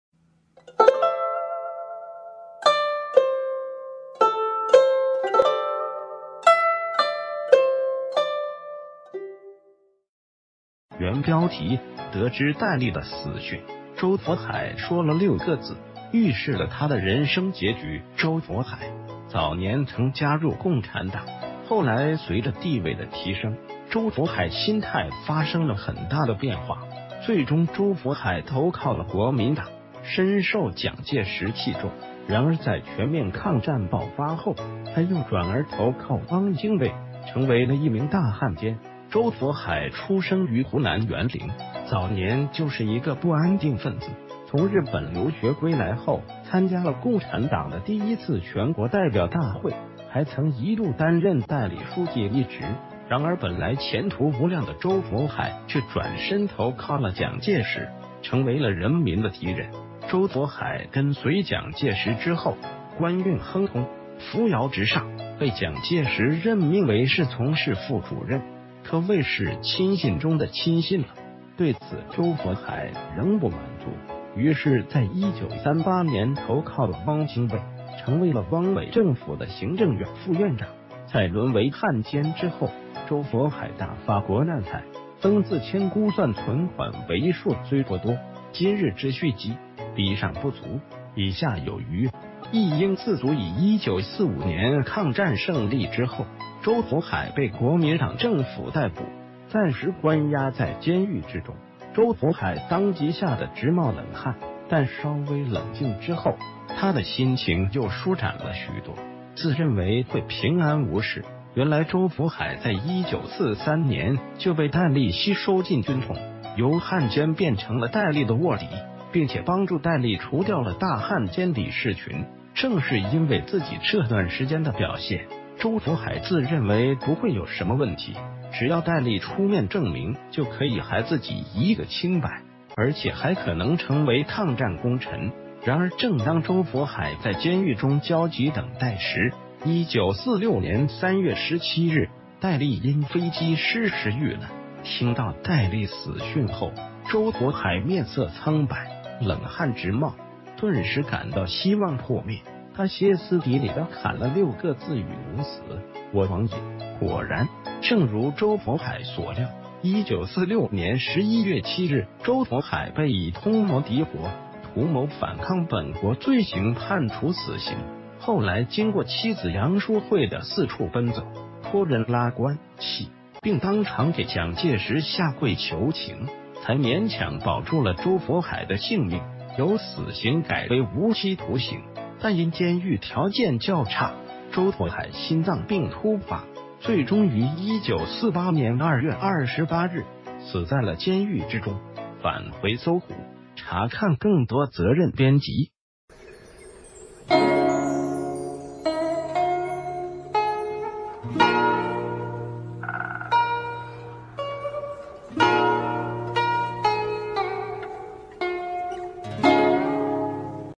得知戴笠的死訊，周佛海說了6個字，預示了他的人生結局_搜狐歷史_搜狐網
原標題：得知戴笠的死...